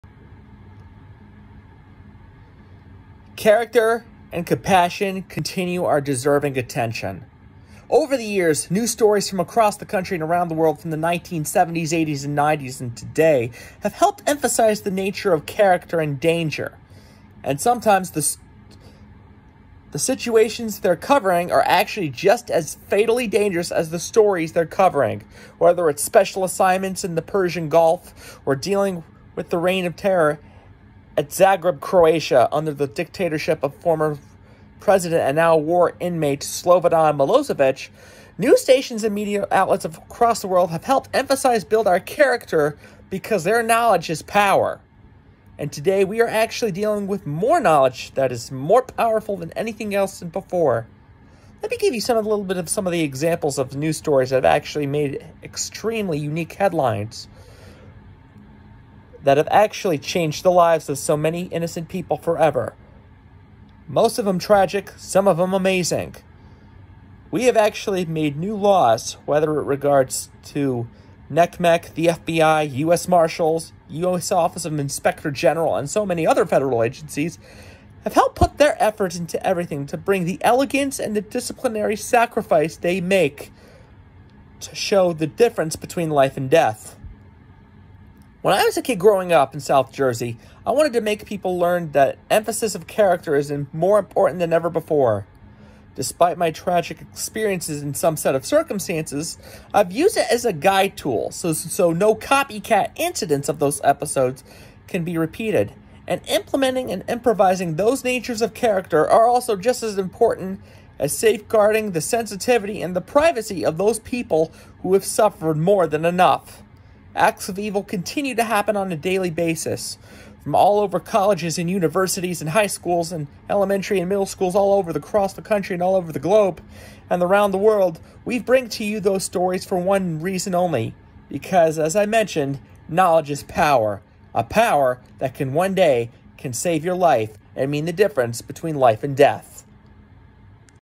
Why news coverage in areas are more important than ever
News coverage because of attitude, courage, compassion, determination, and sensitivity along with elegance and improvising and emphasizing the urgent nature of dangers have shared their experiences so no copycat episodes or incidents can detonate anger in your very own back or front yard. It’s one thing to avoid controversial topics like (racial conflict, religious inequalities or political imbalances) but don’t hold them to a lower standard. They are equally as important as the families they are supporting. Bad behavior is equally terrible and worthless. Let’s focus on the victims and demolish the attackers. Don’t make the stories about the suspects. Make them forgotten. Focus on the victims and their courage to come forward to show you that knowledge is power.